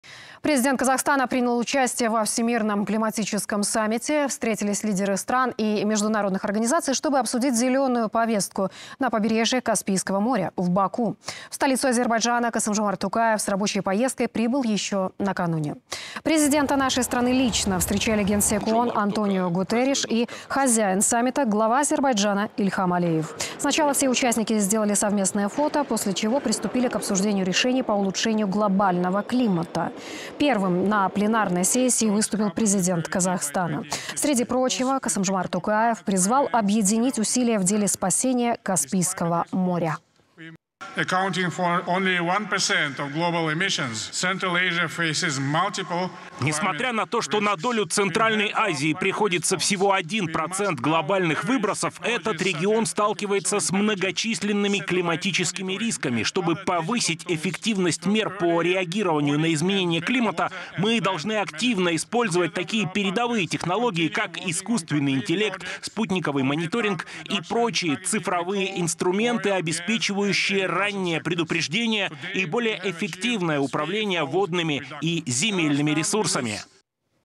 Токаев первым выступил на пленарной сессии климатического саммита в Баку
Президент Казахстана принял участие во Всемирном климатическом саммите. Встретились лидеры стран и международных организаций, чтобы обсудить «зелёную» повестку, на побережье Каспийского моря, в Баку. В столицу Азербайджана Касым-Жомарт Токаев с рабочей поездкой прибыл еще накануне.